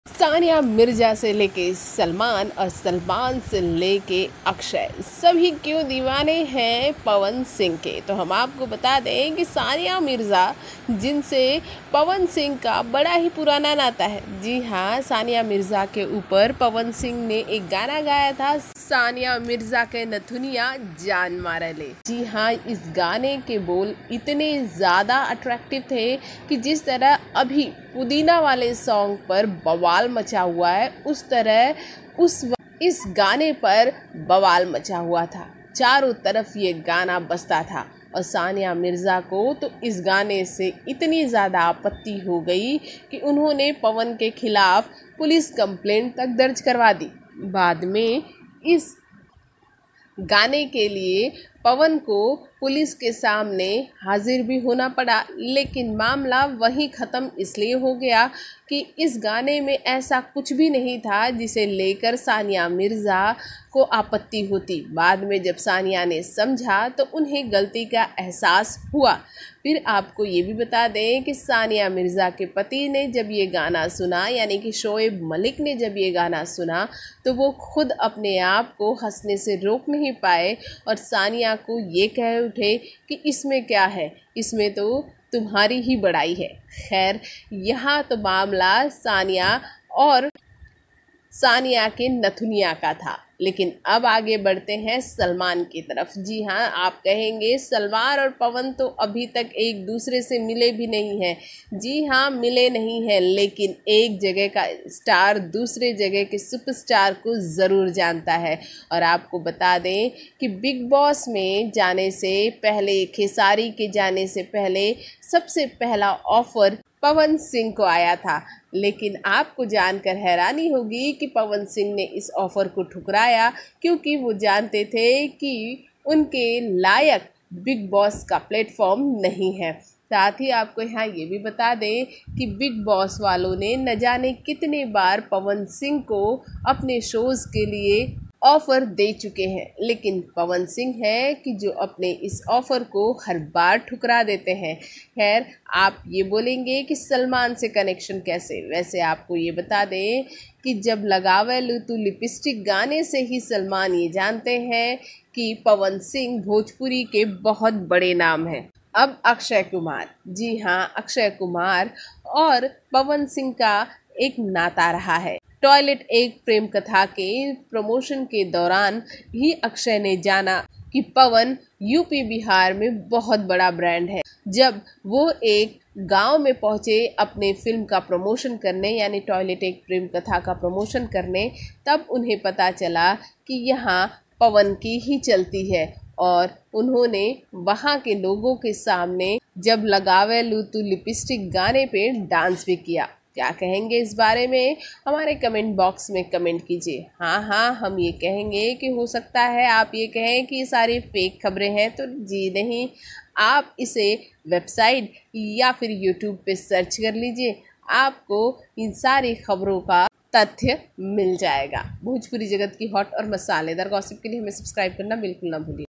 सानिया सलमान और अक्षय का जीना कैसे हराम कर रखा है पवन सिंह ने ।
सानिया सलमा और अक्षय का जीना कैसे हराम कर रखा है पवन सिंह ने ।